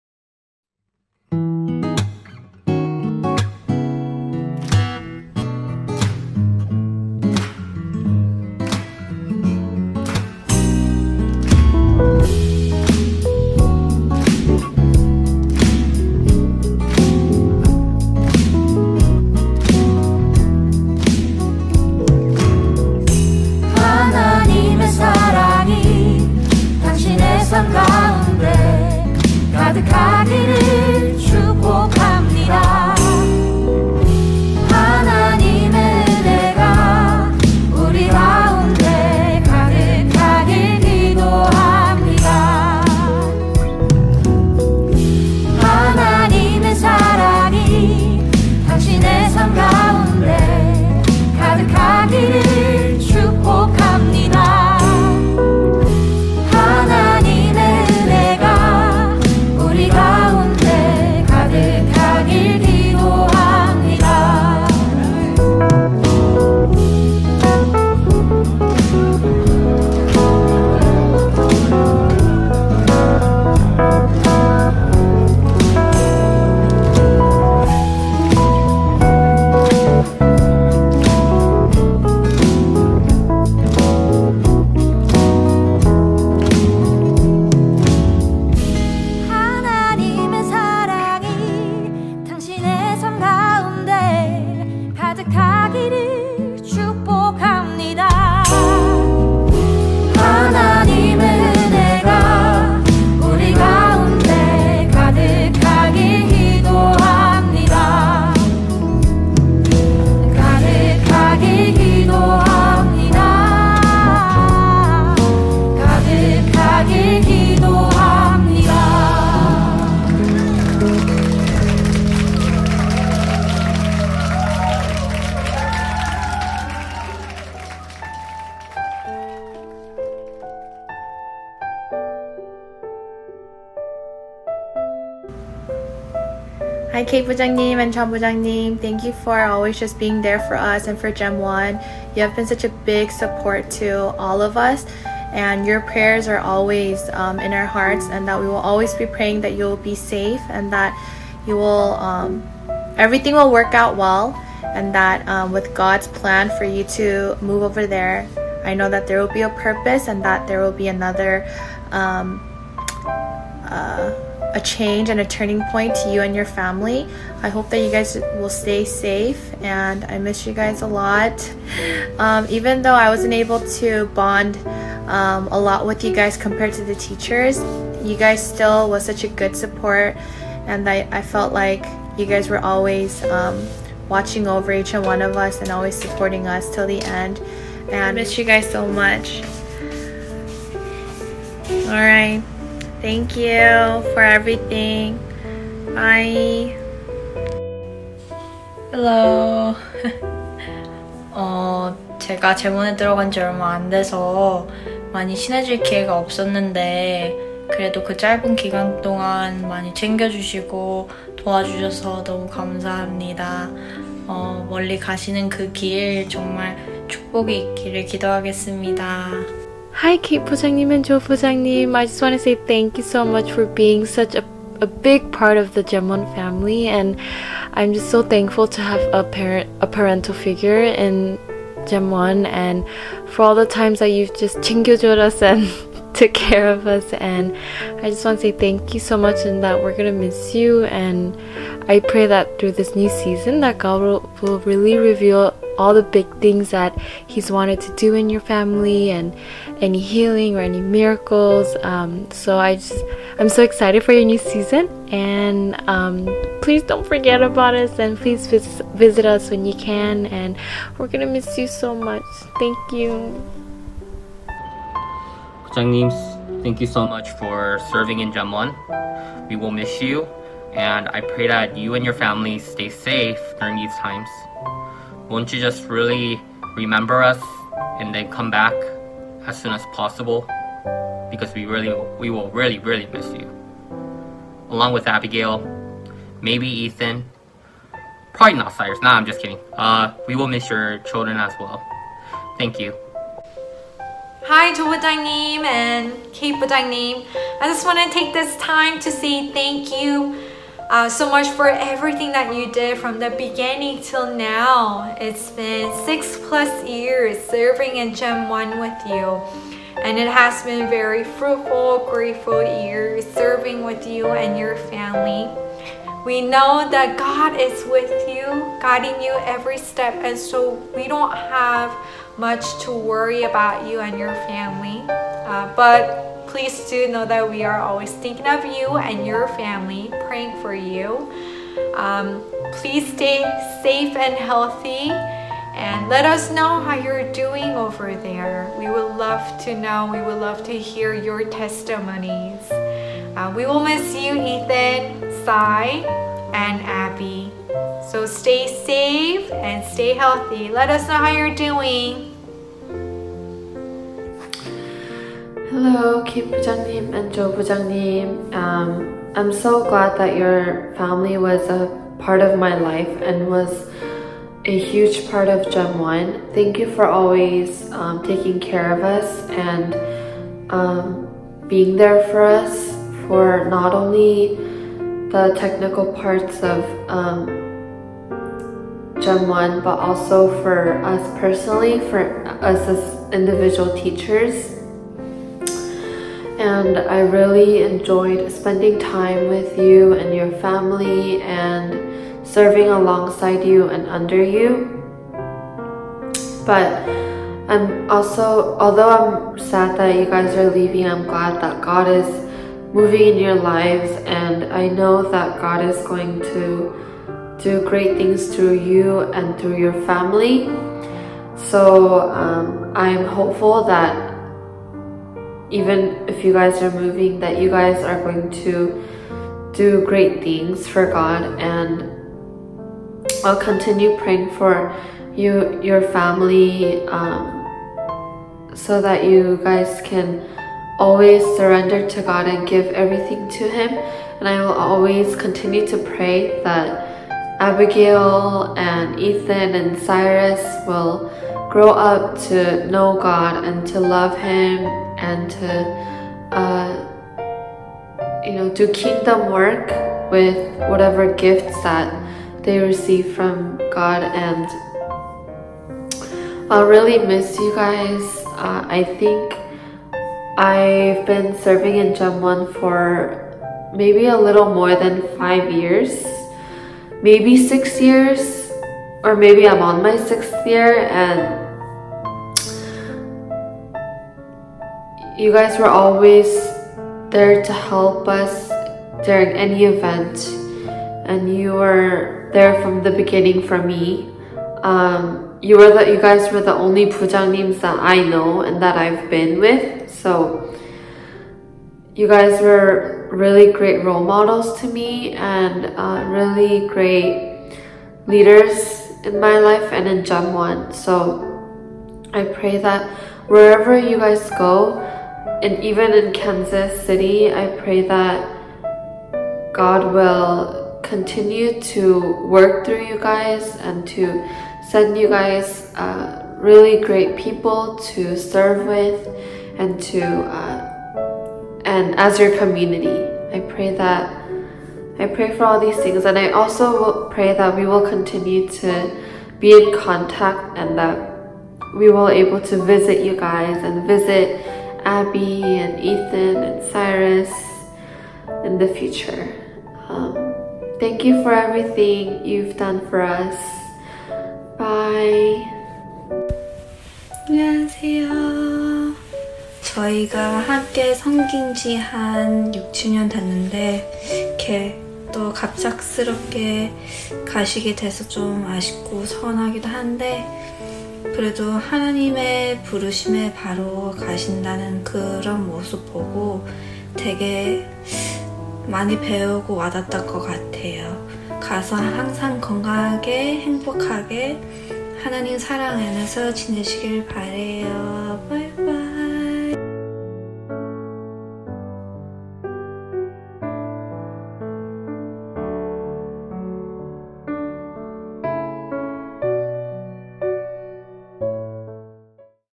JOE & KATE BJN GEM1 2020
Thank you Joe and Kate BJN for everything!
It has been amazing years serving with you in GEM1.
We will dearly miss you and your family.
Keep us in your prayers! We will also keep you in our prayers.
Thank you and love you BJN!